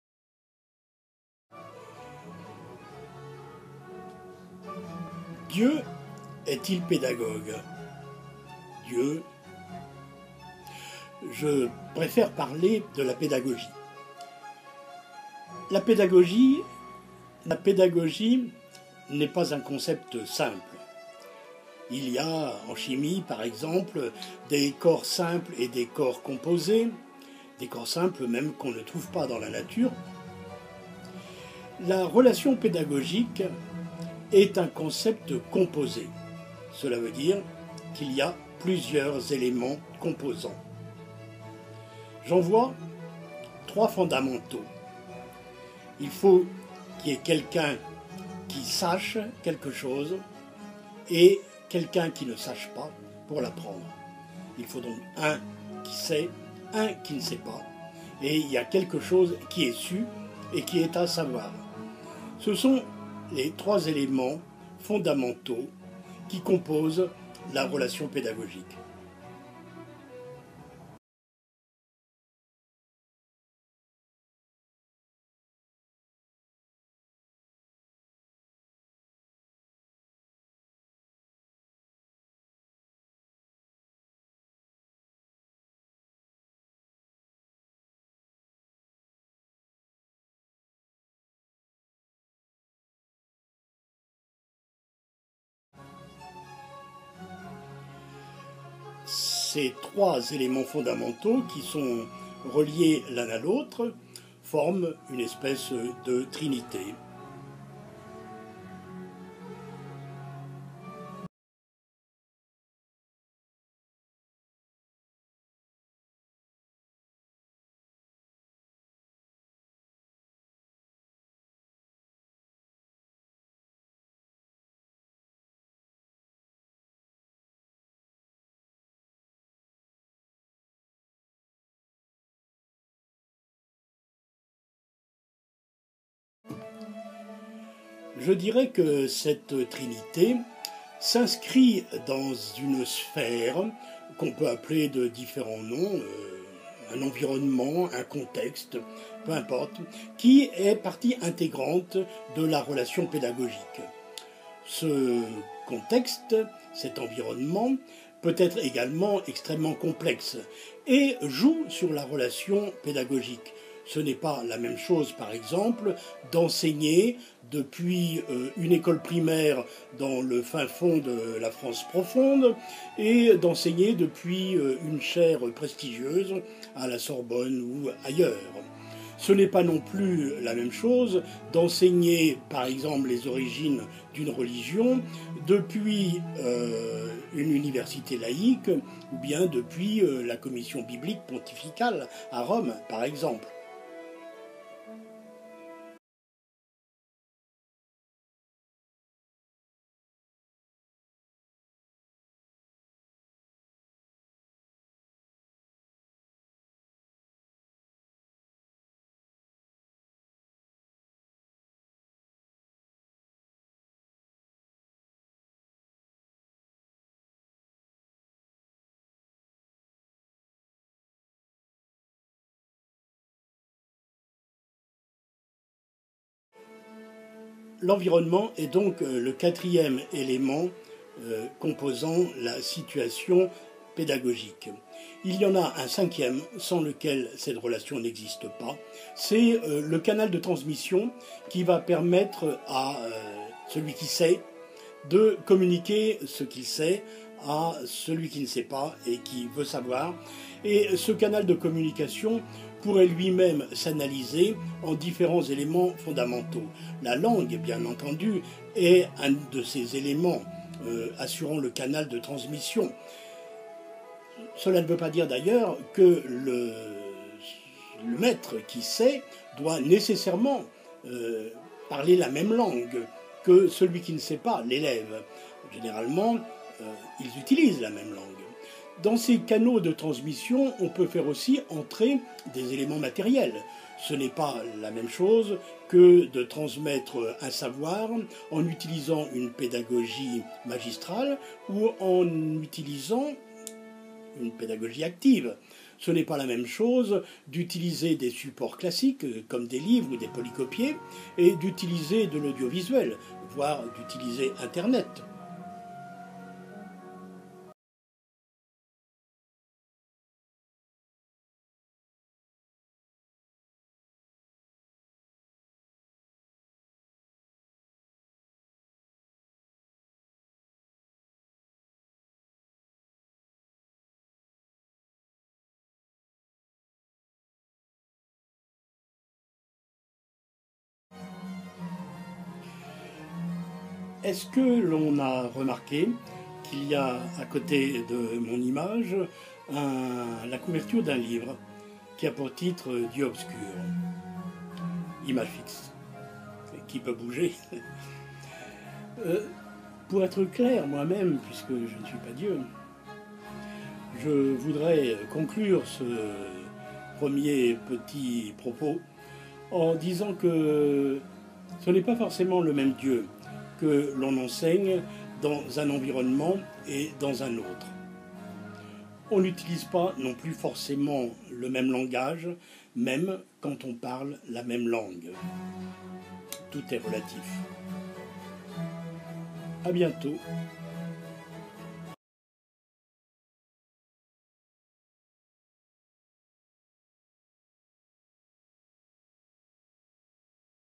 Dieu pédagogue (1).m4v
quelques propos sur le livre "Dieu obscur" (Thomas Römer).